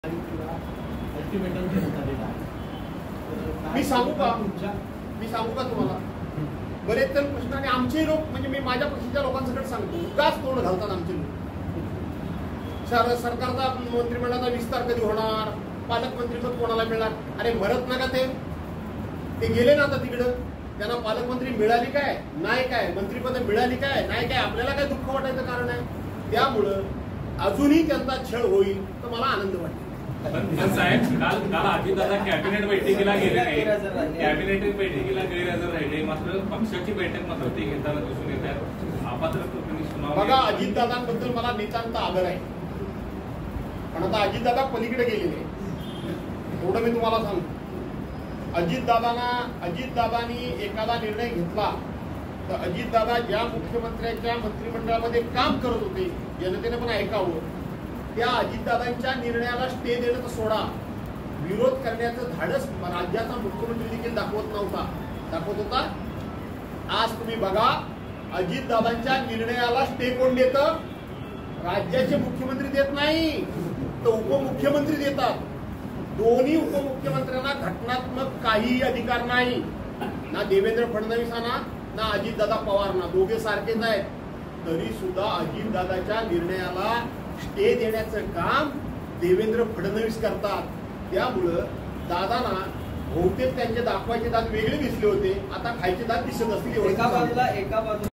Bhaskar Jadhav on Sanjay Raut | शिंदे गटासह राऊतांवरही निशाणा; भास्कर जाधवांनी नाव न घेता सुनावलं
आमची लोक उगाच तोंड घालतात, असं म्हणत भास्कर जाधवांनी नाव न घेता संजय राऊतांना सुनावलं आहे. भास्कर जाधव आज (४ ऑक्टोबर) कोल्हापुरात माध्यमांशी बोलत होते. मंत्री मंडळ विस्तार कधी होणार, पालकमंत्री कोण? याबाबत उगाच आमचे लोक चर्चा करतात. उगाच तोंड घालतात, असं विधान भास्कर जाधवांनी केलं आहे. तसेच, अजितदादांबद्दल मला नितांत आदर पण ते पलीकडे गेले आहेत, असंही जाधव म्हणाले. तसेच, एकनाथ शिंदे कोण? अजितदादा काय? हे मूल्यमापन भाजपने केल्याचा इशाराही जाधवांनी दिला.